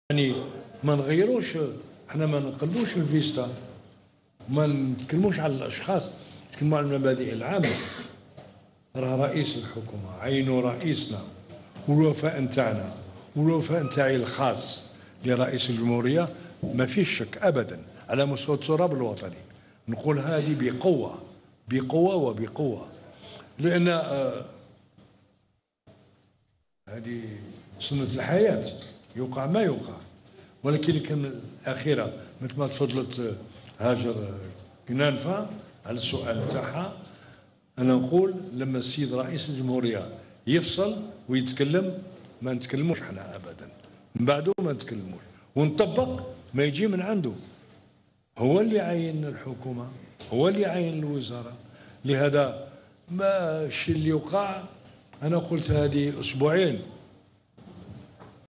جمال ولدعباس الأمين العام لجبهة التحرير الوطني الرئيس بوتفليقة يدعم عبد المجيد تبون
ولدعباس  الأمين العام لجبهة التحرير الوطني الرئيس بوتفليقة يدعم عبد المجيد تبون
الشاب فيصل الصغير nadir cheb houssem cheb djalil way way tawtaw  عبد المجيد تبون سيدي السعيد علي حداد hicem smati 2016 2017